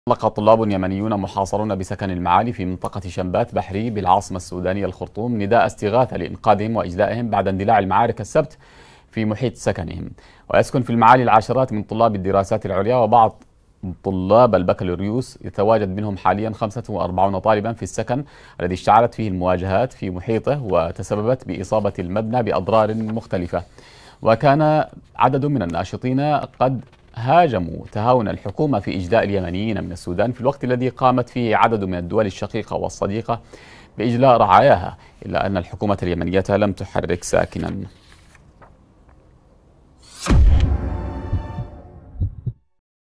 طلاب اليمن يطلقون نداء استغاثة بعد حصارهم في سكنهم بالخرطوم
أطلق طلاب يمنيون محاصرون بسكن المعالي في منطقة شمبات بحري بالعاصمة السودانية الخرطوم
نداء استغاثة لإنقاذهم وإجلائهم بعد اندلاع المعارك السبت، في محيط سكنهم.. ويسكن في المعالي العشرات من طلاب الدراسات العليا وبعض طلاب البكالوريوس يتواجد منهم حاليا خمسة وأربعون طالبا في السكن الذي اشتعلت المواجهات في محيطه وتسببت بإصابة المبنى بأضرار مختلفة... وكان قد هاجم عدد من الناشطين تهاون الحكومة في إجلاء اليمنيين من السودان في الوقت الذي قامت عدد من الدول الشقيقة والصديقة بإجلاء رعاياها إلا أن الحكومة اليمنية لم تتحرك ساكنا

تليجرام